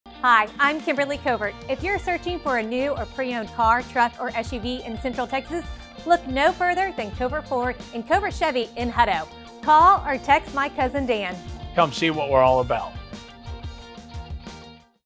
If you’re searching for a new or preowned vehicle in Central Texas, look no further than Covert!
Hi, I’m Kimberley Covert.

If you’re searching for a new or preowned car, truck or SUV in Central Texas, look no further than Covert Ford and Covert Chevy in Hutto.

Come in today for a test drive and experience the Covert Commitment to Excellence.